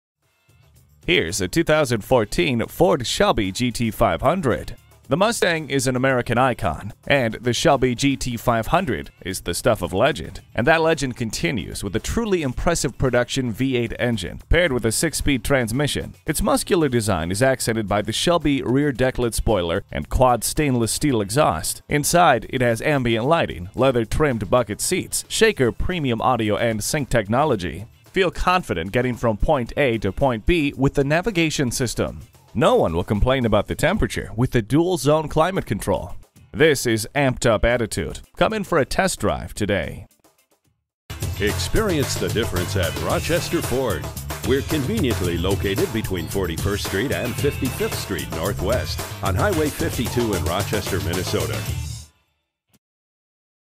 2014 Ford Mustang Rochester MN Winona, MN FA158452 - SOLD
Year: 2014
Make: Ford
Model: Mustang
Trim: Shelby GT500
Engine: 5.8L V8 32V Supercharged
Transmission: Tremec 6-Speed Manual
Color: Black
Interior: Charcoal Black
Mileage: 4321
Stock #: FA158452
VIN: 1ZVBP8JZ2E5263274
2014 Ford Mustang Shelby GT500, 2D Coupe, 5.8L V8 32V Supercharged, Tremec 6-Speed Manual, RWD, Black. Autocheck One Owner, No Accidents, Local Trade and Bluetooth. Equipment Group 821A, Navigation System Package (Dual Zone Automatic Temperature Control), SVT Performance Package (SVT Engineered Bilstein Cockpit Selectable Dampers and TORSEN Differential w/3.31 Axle Ratio), Charcoal Black w/Red Accents w/Heated Leather Sport Bucket Seats, 19" x 9.5" Fr/20" x 9.5" Rr Painted Wheels, 4-Wheel Disc Brakes, 6-Way Power Driver's Seat, ABS brakes, Air Conditioning, Electronic Stability Control, External Engine Oil Cooler, Front Bucket Seats, Front Center Armrest, Front fog lights, Heated front seats, High-Intensity Discharge Headlights, Navigation System, Rear window defroster, Remote keyless entry, Security system, Shaker Pro Audio System w/9 Speakers, Speed control, Split folding rear seat, Steering wheel mounted audio controls, SVT Track Pack, Tachometer, Tilt steering wheel, Traction control, and Transmission Cooler w/Pump. We look forward to earning your business.

Address:
4900 Hwy 52 North